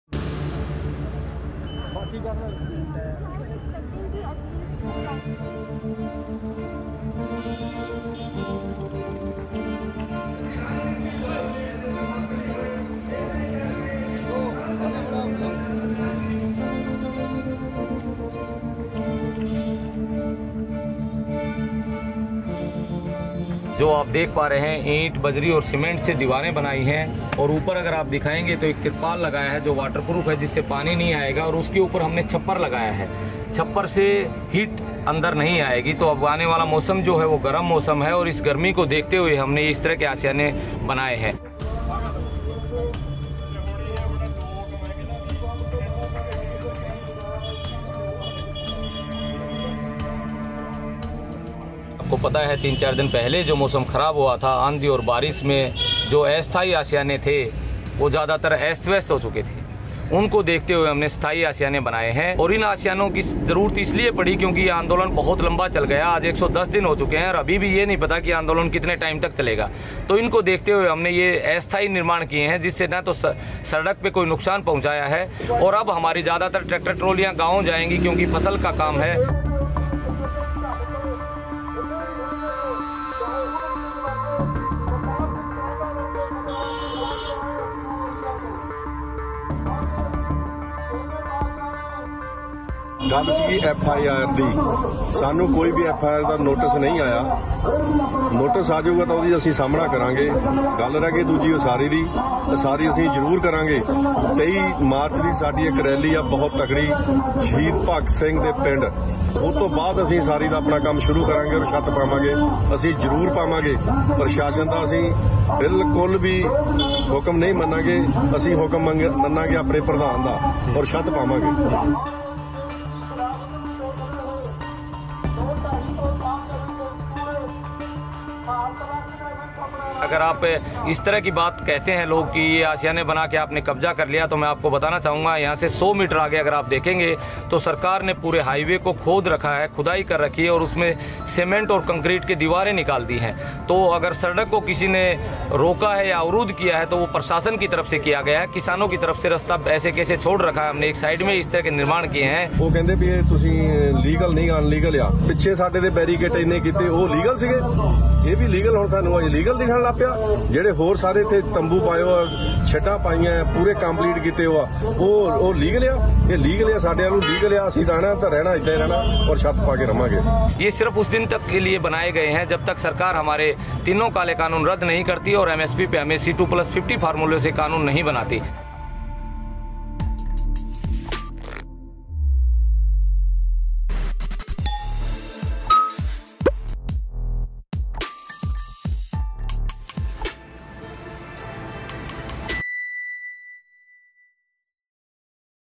Farmers protest: गर्मी के मौसम को झेलने की किसान कर रहे तैयारी | Quint Hindi
सरकार और किसानों के बीच कृषि बिल को लेकर गतिरोध जारी है,, टिकरी बॉर्डर (Tikri Border) पर कड़ाके की ठंड का सामना करने के बाद किसान गर्मी के मौसम को झेलने की तैयारी कर रहे हैं. अस्थायी घरों का निर्माण कर रहे हैं.